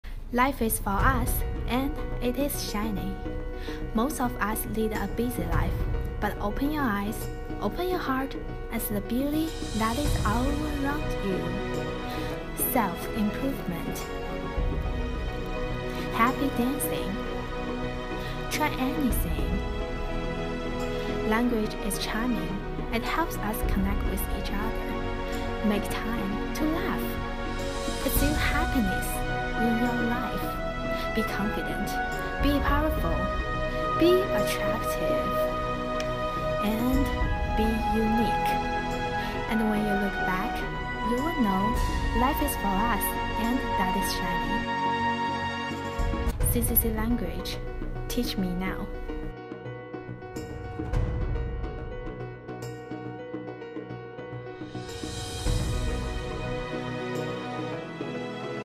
Dance with Joy
This is an ad I created for CCC Language (Chinese Coffee Class) back in April 2018. Unfortunately, to my knowledge, it was never published, and the business has since closed its doors. This project was filmed in Bellevue, Washington, just outside Seattle. I had a great time working on this video and wanted to share it with you all as a part of my creative journey.